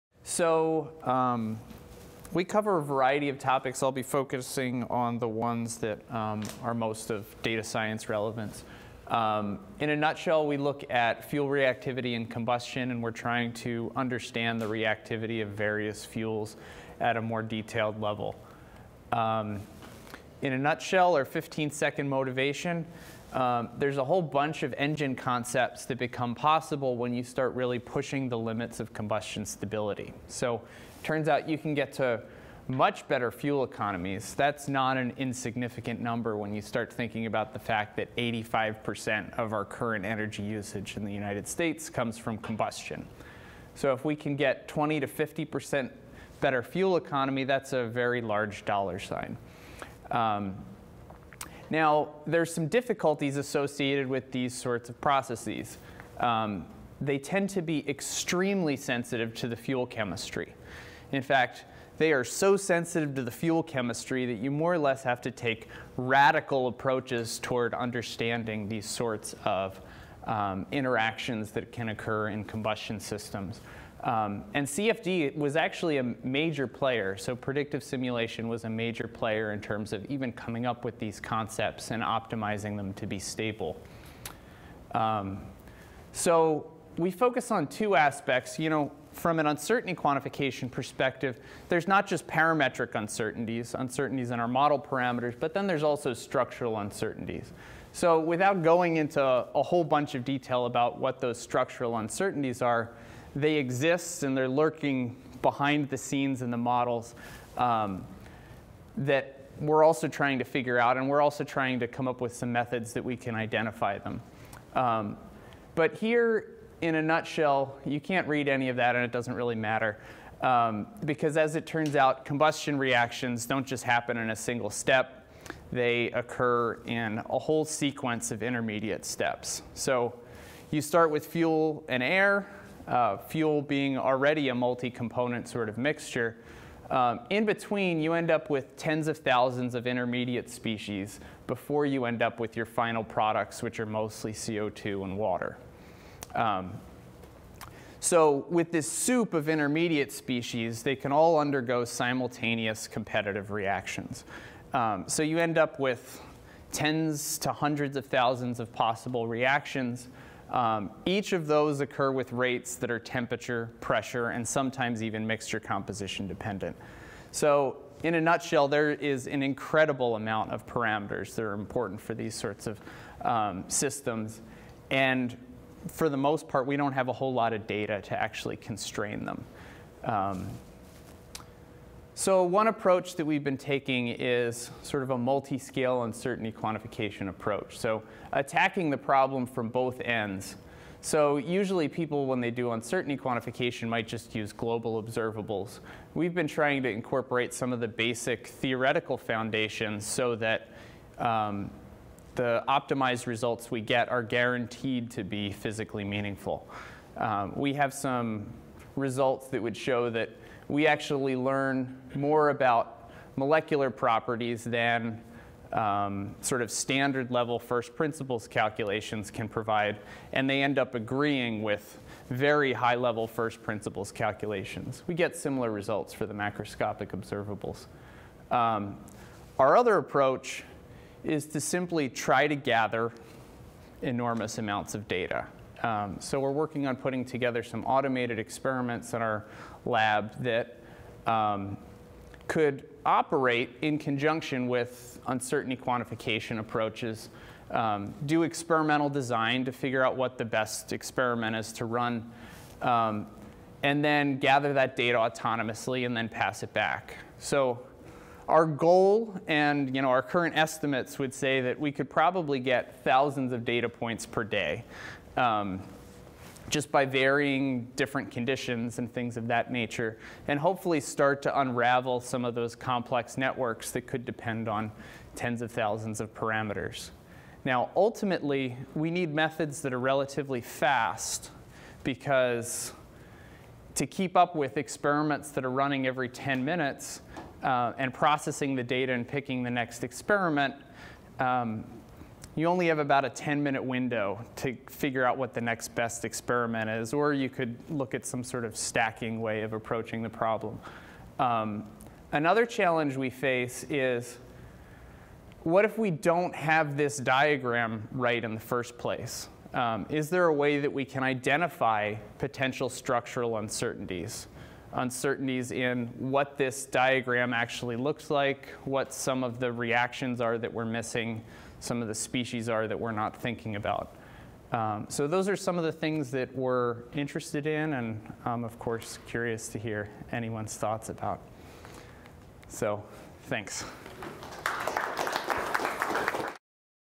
Speed-Networking Seminar | Michael Burke : Automated, High-Throughput Experiments
Michael Burke, Assistant Professor in Mechanical Engineering at Columbia University